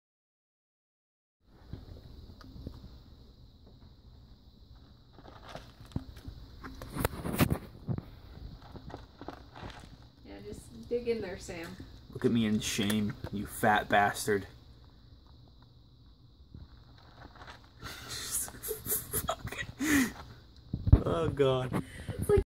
Sam the Cat eats like a shovel
Man he hungry, eating like a god damn shovel.